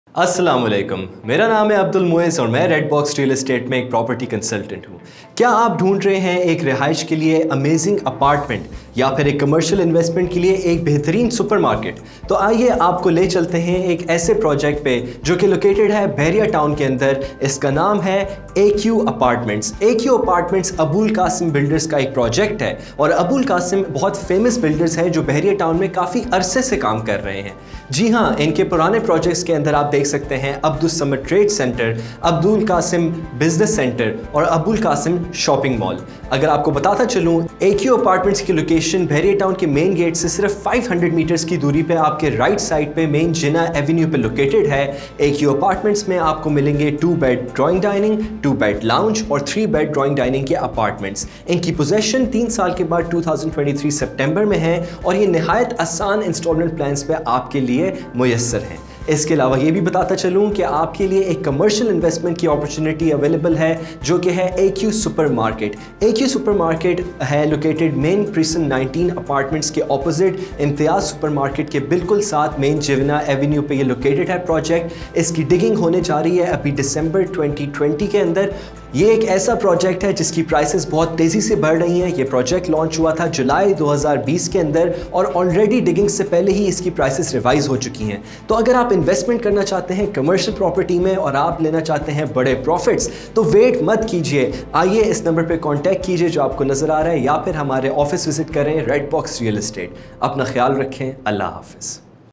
WHERE TO INVESTMENT IN BAHRIA TOWN KARACHI |BTK BENEFICIAL PROJECT | COMMERCIAL|RESIDENTIAL PROPERTY
In this video, you will get the answer to all your questions that where to invest in Bahria Town Karachi.